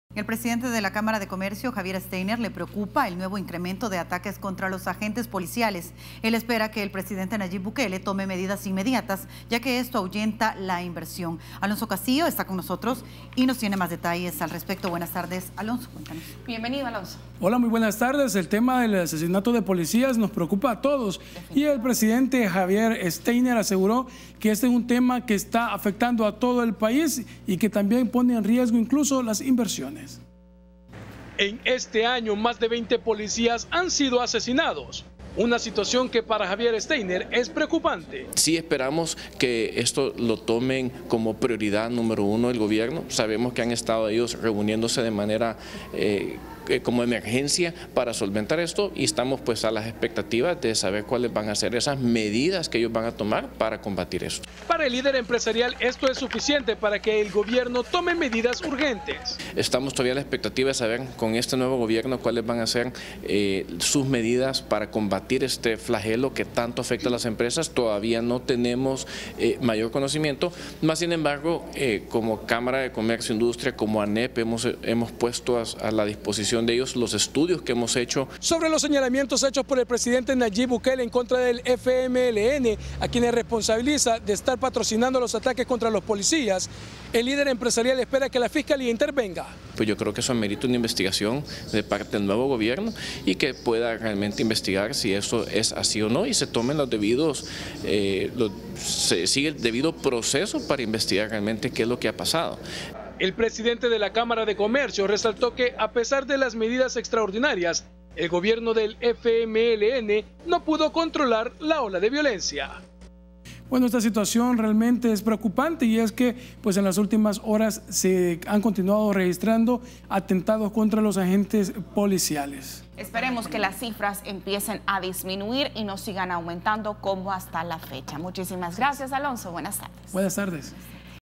Cámara de Comercio preocupada por asesinato de policías
Al presidente de la Cámara de Comercio, Javier Steiner, le preocupa el nuevo incremento de ataques contra los agentes policiales.

Él espera que el presidente Nayib Bukele tome medidas inmediatas, ya que esto ahuyenta la inversión.